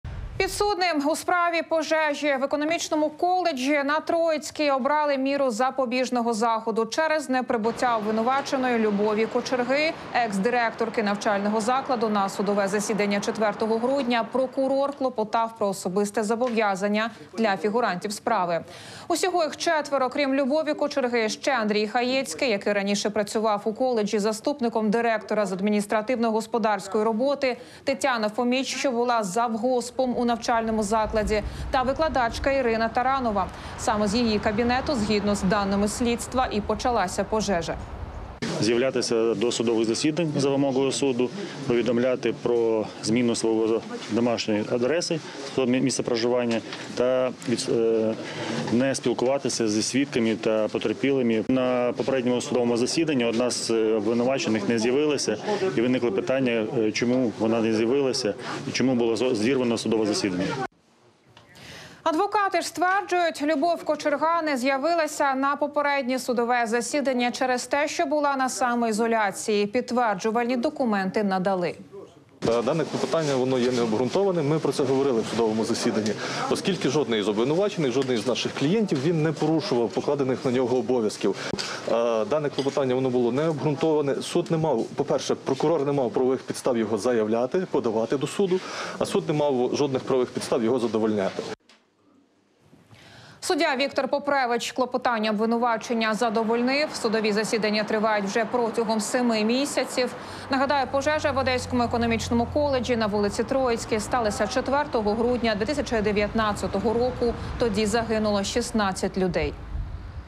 Пожежа на Троїцькій: підозрюваним обрали запобіжний захід
Підсудним у справі пожежі в економічному коледжі на Троїцькій обрали міру запобіжного заходу.
Через неприбуття обвинуваченої Любові Кочерги, ексдиректорки навчального закладу, на судове засідання 4 грудня, прокурор клопотав про особисте зобов’язання для фігурантів справи. Усього їх четверо: крім Любові Кочерги, ще Андрій Хаєцький, який раніше працював у коледжі заступником директора з адміністративно-господарської роботи, Тетяна Фоміч, що була завгоспом у навчальному закладі, та викладачка Ірина Таранова. Саме з її кабінету, згідно з даними слідства, і почалася пожежа.

«З’являтися до судових засідань на вимогу суду, повідомляти про зміну своєї домашньої адреси (місця проживання) та не спілкуватися зі свідками та потерпілими. На попередньому судовому засіданні одна з обвинувачених не з’явилася. І виникли питання, чому вона не з’явилася, чому було зірване судове засідання», — повiдомив прокурор Євген Антощук.

Адвокати ж стверджують: Любов Кочерга не з’явилася на попереднє судове засідання через те, що була на самоізоляції. Підтверджувальні документи надали.

«Це клопотання є необґрунтованим, ми про це говорили на судовому засіданні. Оскільки жодний з обвинувачених, жодний з наших клієнтів, він не порушував покладених на нього обов’язків. Клопотання було необґрунтоване: прокурор не мав, по-перше, його подавати до суду, а суд не мав жодних правових підстав його задовольняти», — заявив адвокат Ярослав Мартинюк.

Суддя Віктор Попревич клопотання обвинувачення задовольнив. Судові засідання тривають вже протягом семи місяців.

Нагадаємо, пожежа в Одеському економічному коледжі на вулиці Троїцькій сталася 4 грудня 2019 року. Тоді загинуло 16 людей.